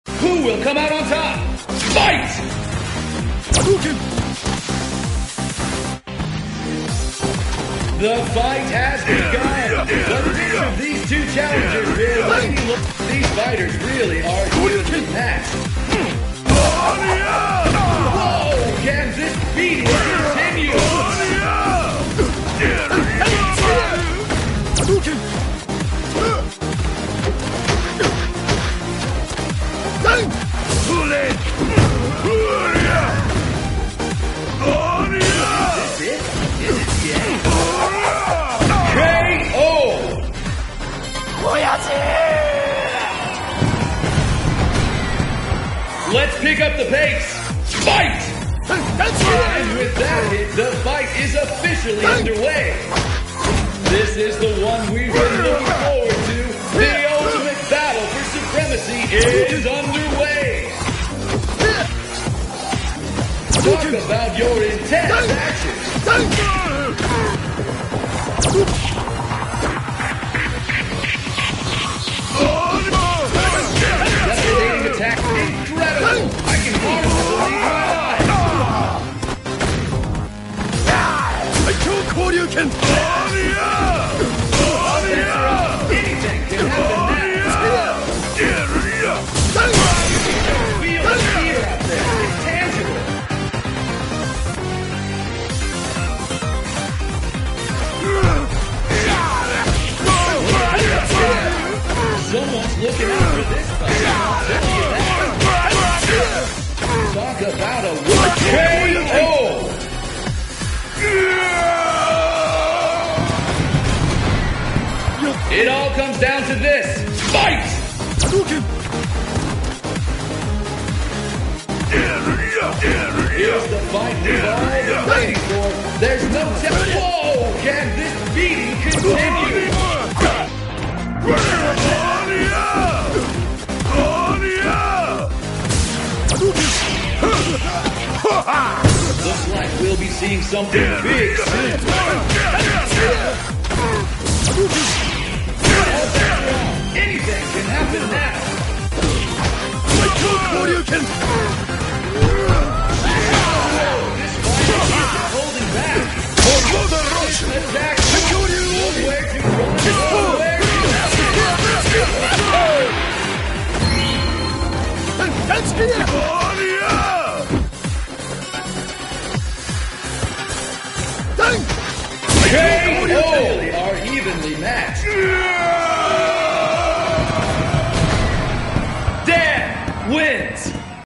Diary 3: unknown player (Zangief) vs. Hibiki-san (Dan)
single match:
Dan vs. Zangief

unknown player (Zangief) vs. Hibiki-san (Dan).